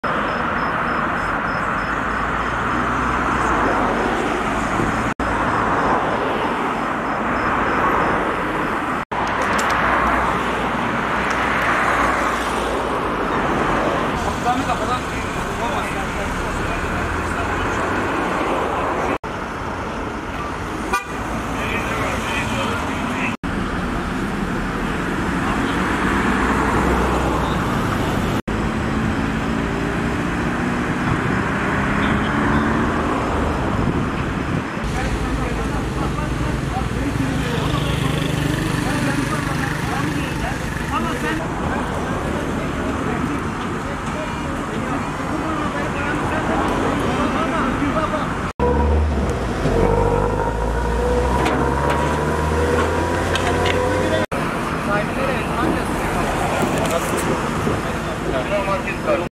Motosiklet yayaya çarptı! Ölü ve yaralılar var
Elazığ'da motosikletin yayaya çarpması sonucu 1 kişi öldü, 2 kişi yaralandı.